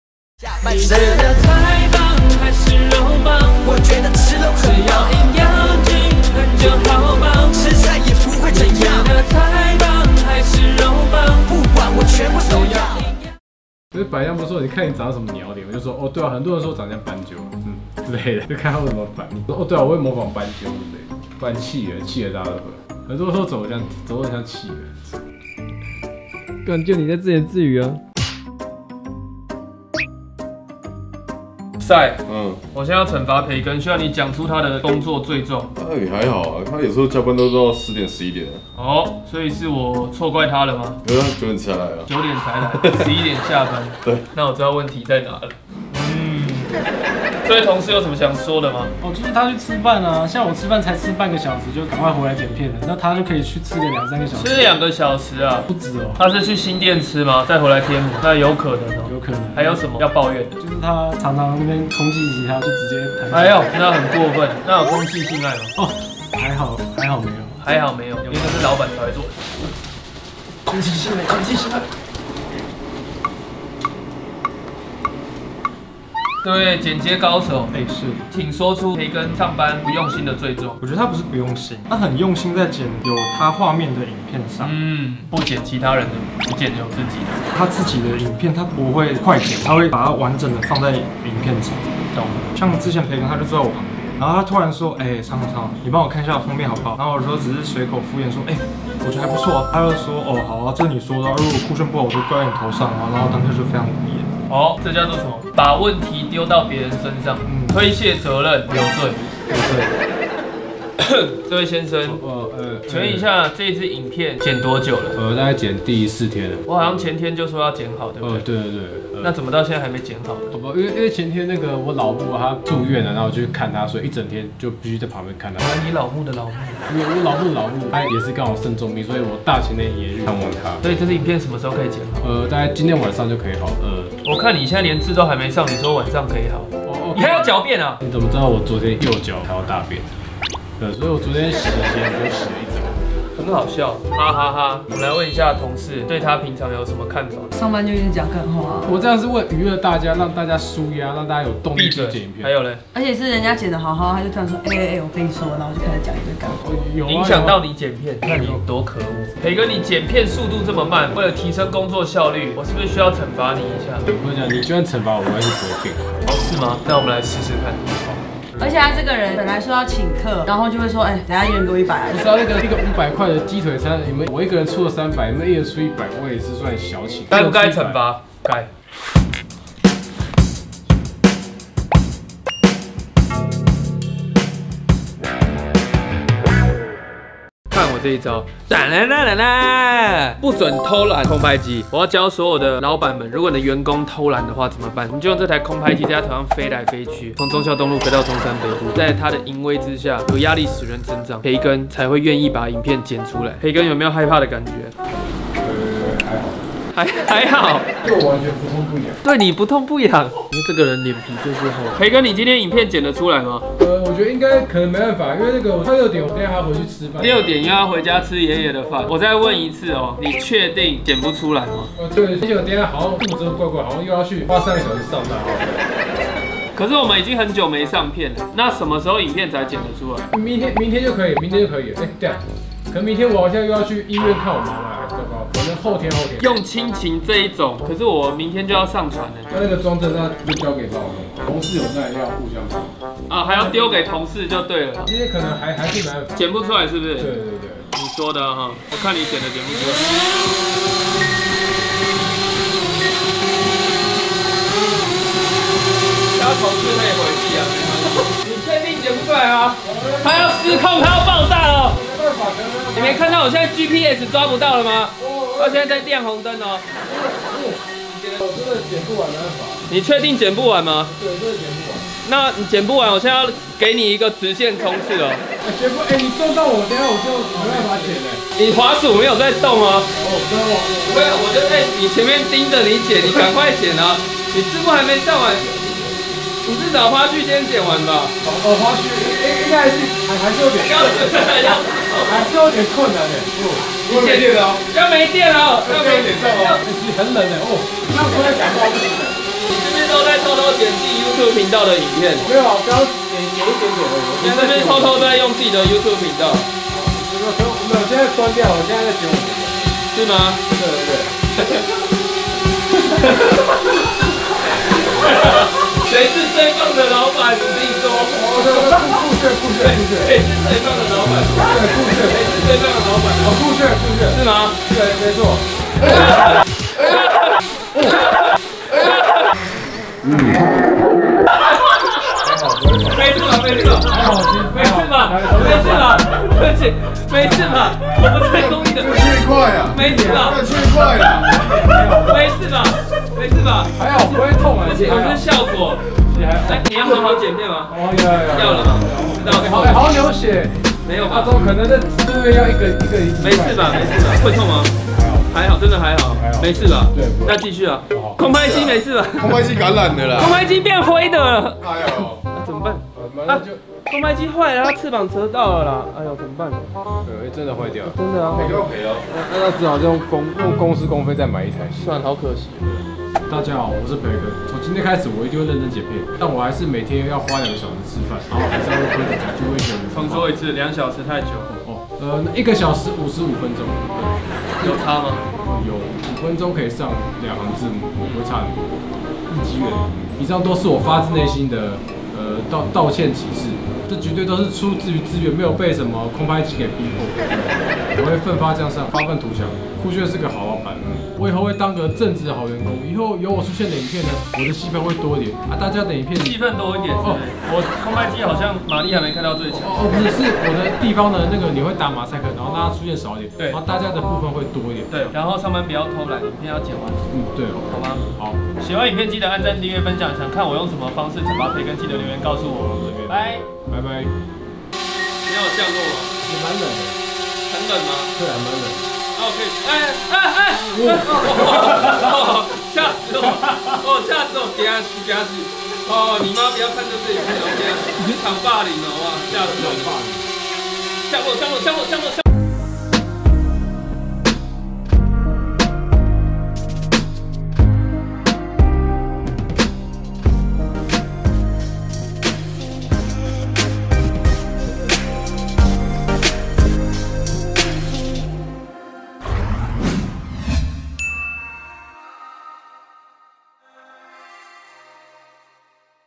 不准偷懶空拍機?!?培根上班騷擾其他同事!!不好好剪片?【WACKYBOYS│反骨男孩】
還想看培根怎麼被惡整或是團員~!記得下面留言喔~記得喔~!!!

wackyboys官網
有感覺娛樂有限公司